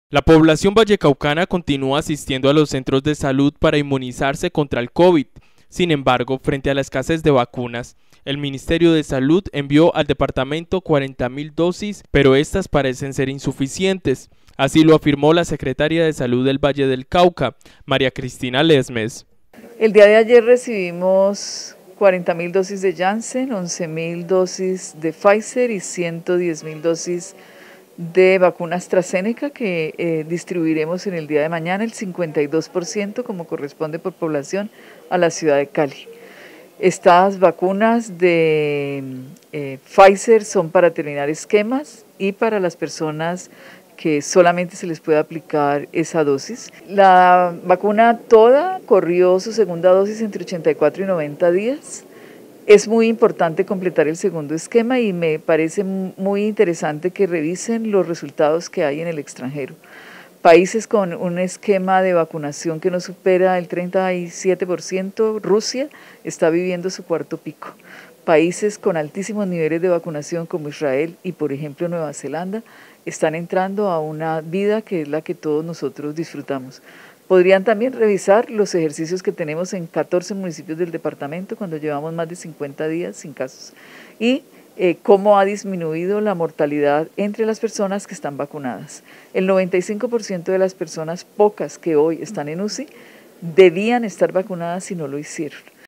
LLEGAN 40 000 VACUNAS AL VALLE DEL CAUCA
La población vallecaucana continua asistiendo a los centros de salud para inmunizarse contra el covid, sin embargo, frente a la escases de vacunas, el ministerio de salud envió al departamento 40 mil dosis más pero estas parecen ser insuficientes, así lo afirmó La secretaria de Salud del Valle del Cauca, María Cristina Lesmes